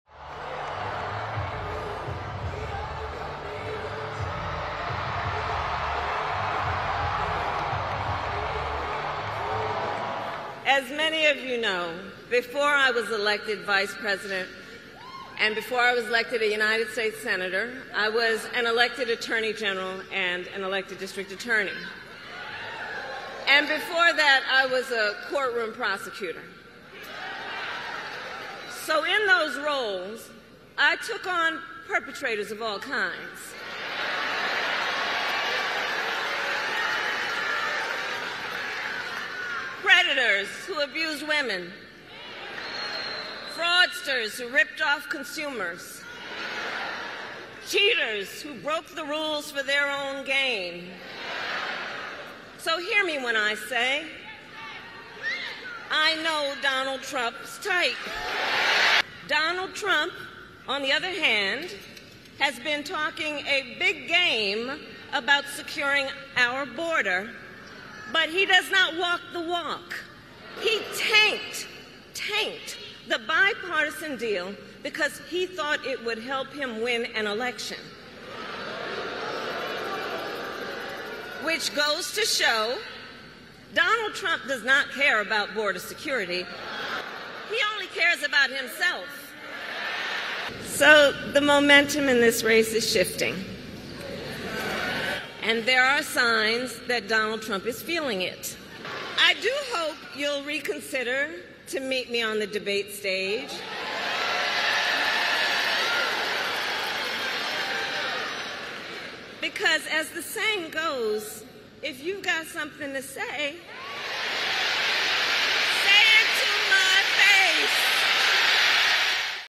'Say it to my face': Kamala Harris challenges Trump to debate her
Vice President Kamala Harris told a cheering, boisterous, packed Atlanta arena on Tuesday that the next 98 days would be a fight, but they’d win come November, as she taunted Donald Trump for wavering on whether he’d show up for their upcoming debate.